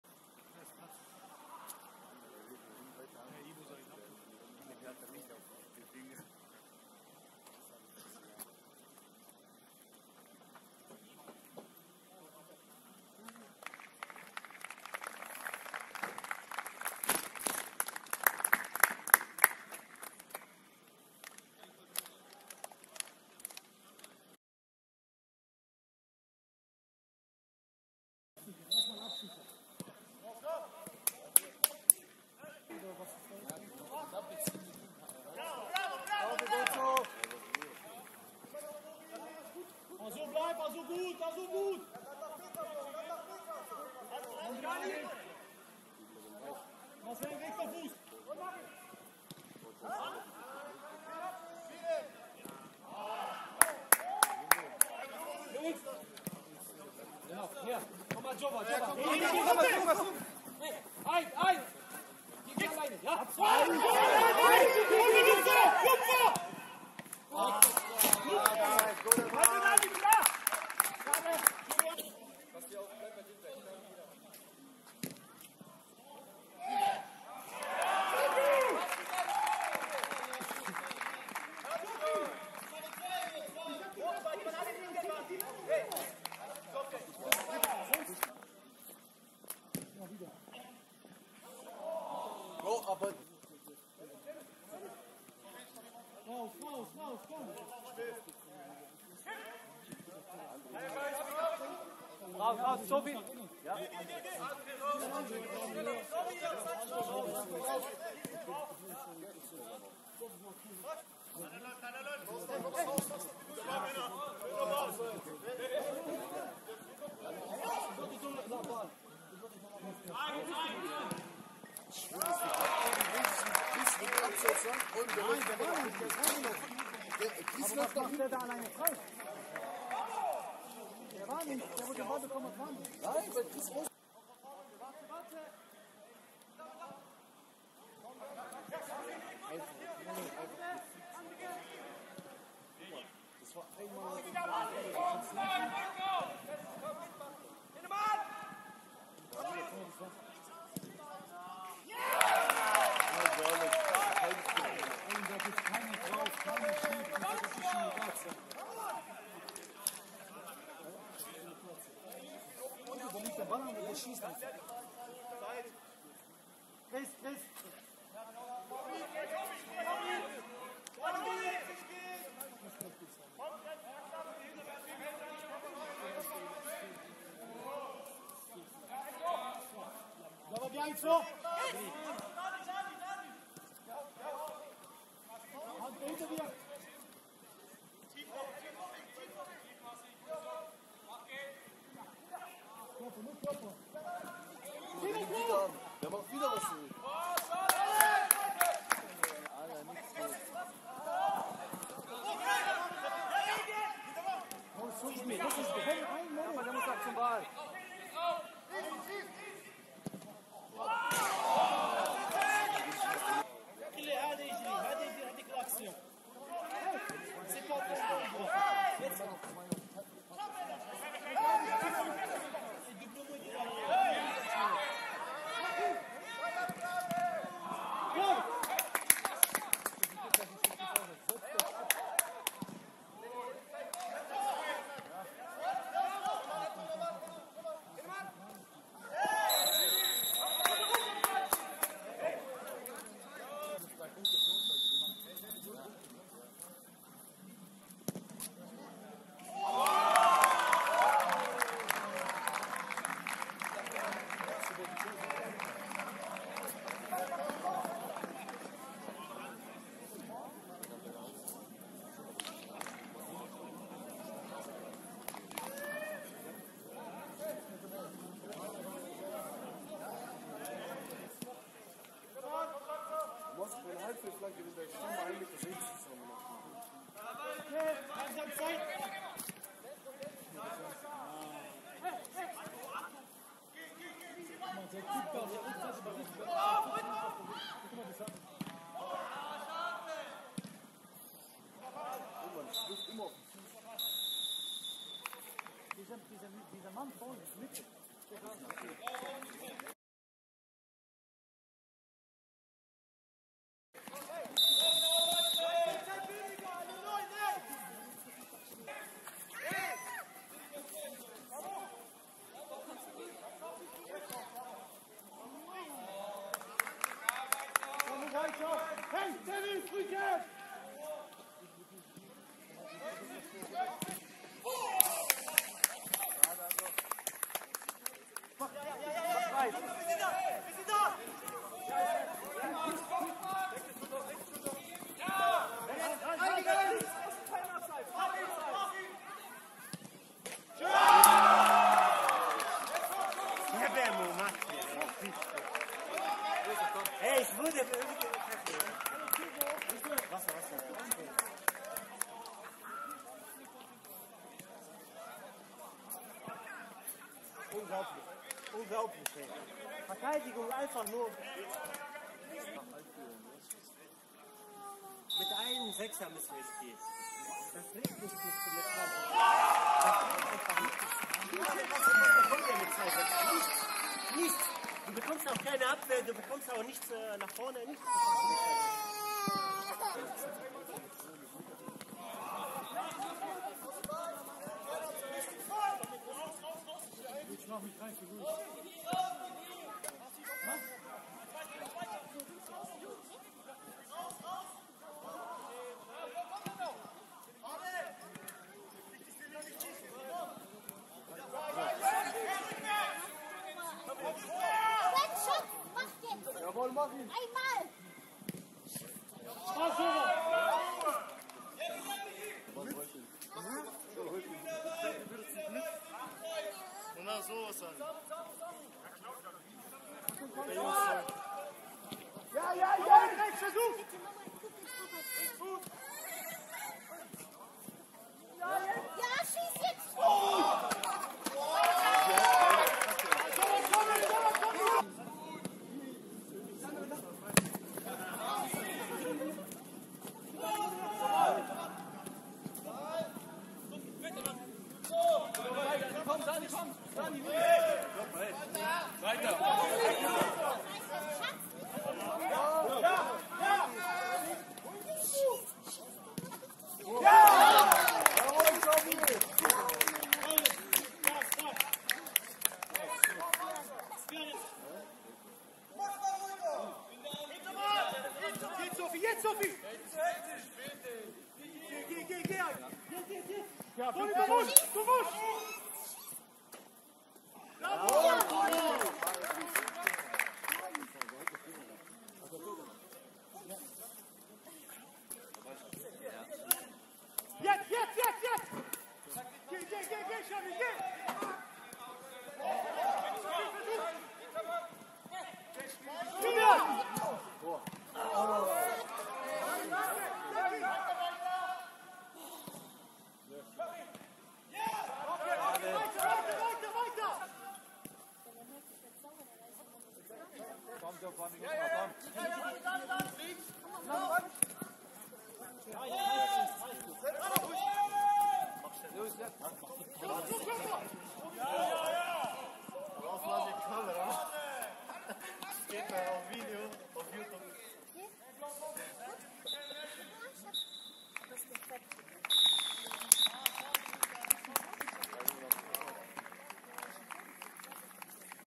PRESAISON SOMMER 2024: TV MÖGLINGEN VS SKV EGLOSHEIM II (22.08.24)
Im 6. Testspiel der Sommervorbereitung ist die Reserve aus Eglosheim zu Gast an der Ludwigsburger Straße.